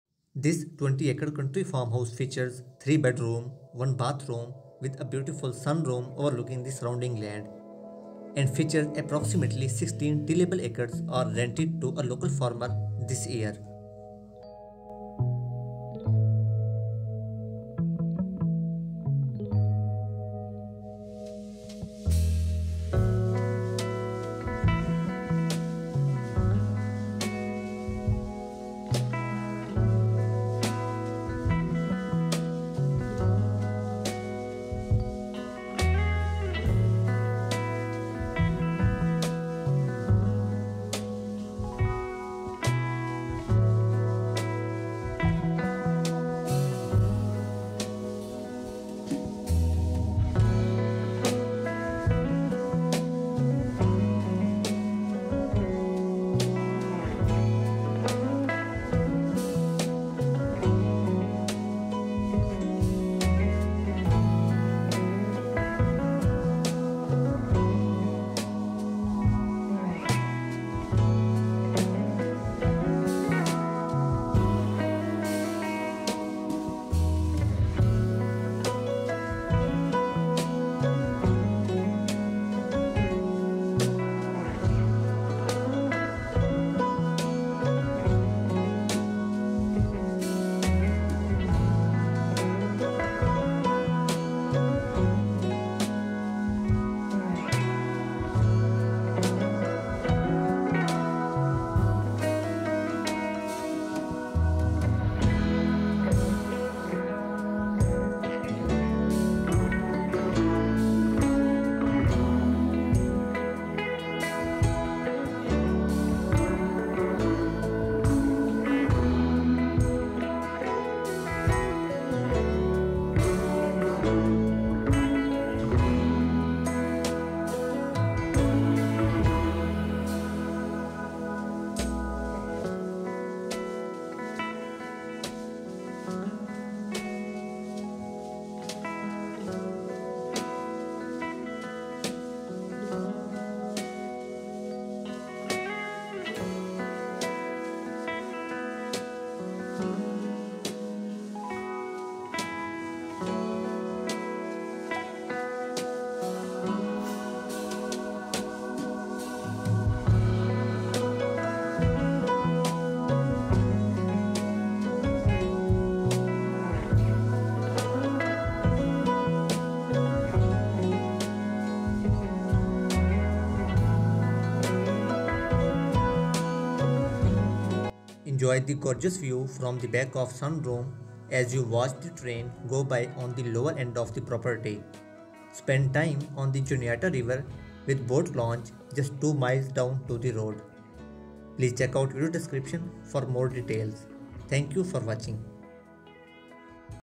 Pennsylvania Farmhouse For Sale | $394k | Pennsylvania Real Estate For Sale | Tillable Land For Sale
$394,000

3beds

1baths

1,292sqft

1057 River Rd, Mc Veytown, PA 17051

Welcome to 1057 River Rd in beautiful McVeytown Pa. Here you will find a 20 acre country farmette featuring a three bedroom, one bathroom home. Enjoy the gorgeous view from the back sunroom as you watch the train go by on the lower end of the property! The small bank barn can be home to animals of your choice. Approximately sixteen tillable acres are rented to a local farmer this year. Spend time on the Juniata River with the McVeytown boat launch just two miles down the road. Call today to schedule your own private showing while there is yet time!